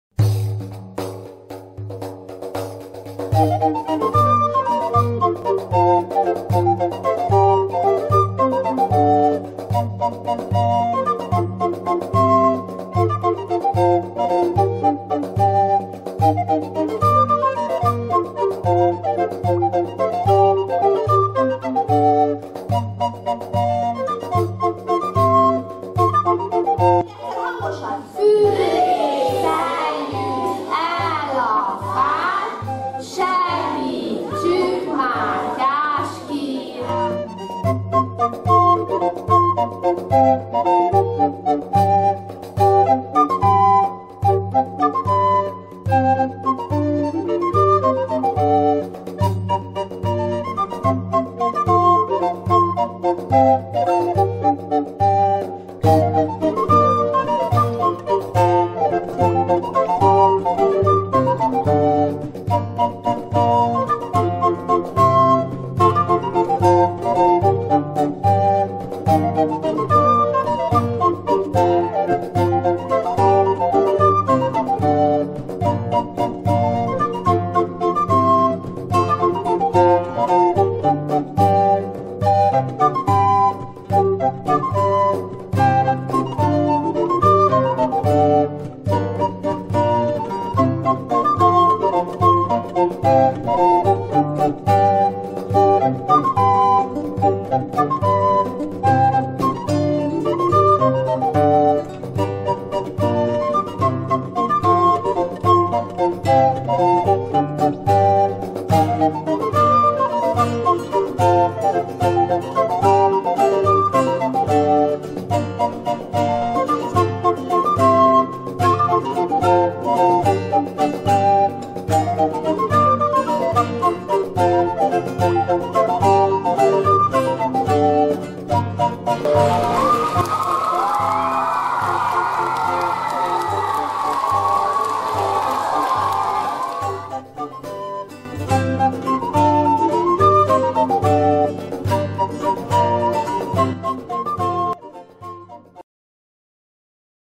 Kuttyomfitty Társulat: Három az igazság! -három mese Mátyás királyról
Ki lesz Mátyás király? Hogyan jár pórul a kolozsvári bíró? Milyenek a furcsa látogatók? Hogy lesz király az igazmondó juhászból?

Nézze a Kuttyomfitty Társulat új műsorát, mely elrepíti a tisztelt nagyérdeműt Mátyás király korába. Az új  interaktív előadás, mely élmény kicsinek és nagynak egyaránt. Pazar díszletek közt, korhű és látványos táncokkal fűszerezve!

Szereplők: Dobsa Fodor Mónika – (tánc, ének, szöveg)
                  Dobsa Tamás – (tánc, ének, szöveg, )

Rendezte: A Kuttyomfitty Társulat
Dramaturg: Dobsa Tamás
Díszlet: Szabó Szlivia és Dobsa Tamás